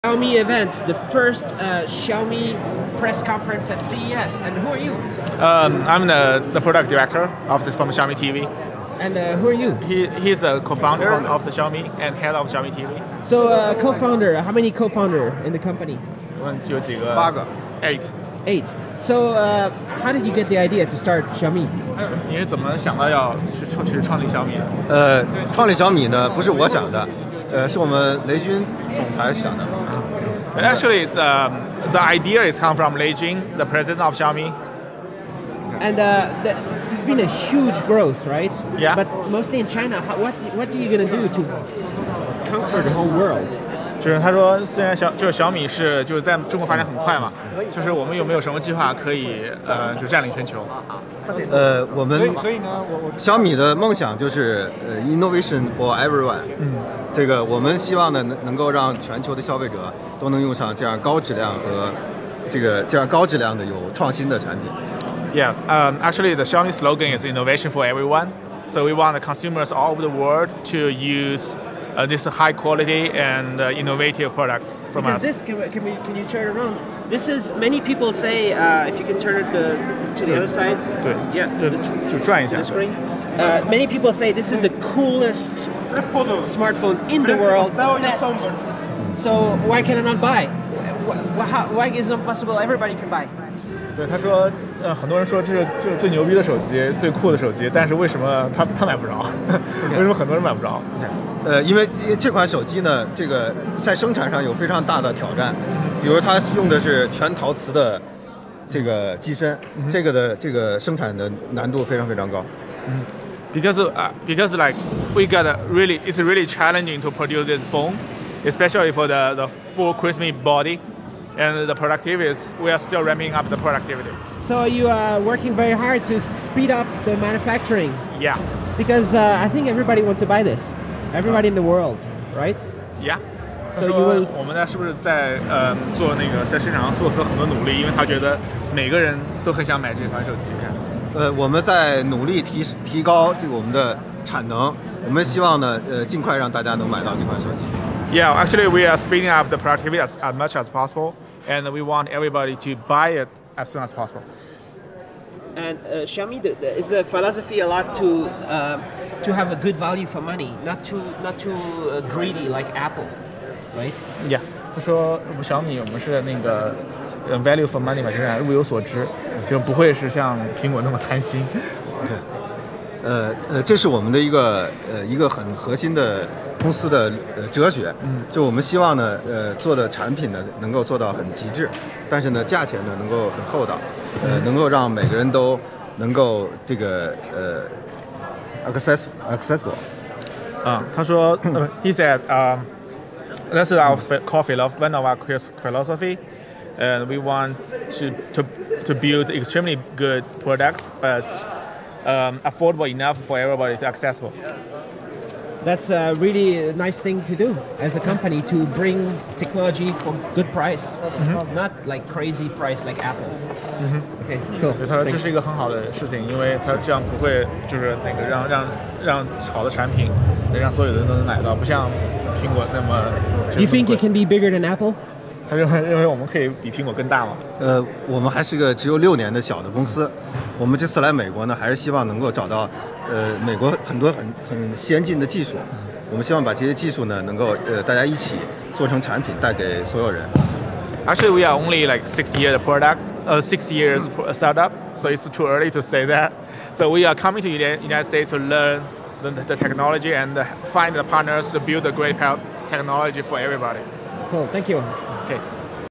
Xiaomi co-founder VP Chuan Wang shows Mi Mix 6.4" bezel-less phone
Xiaomi is a 6-year old startup from China that has shipped hundreds of millions of Smartphones, but mostly in China. Xiaomi is trying to expand to Europe and the USA. Here presenting the Xiaomi Mi Mix that uses a bezel-less 6.4" 1080p Sharp IGZO display, on Qualcomm Snapdragon 821, 6GB RAM, 16Megapixel back camera, 5Megapixel front camera, 128gb or 256gb of storage.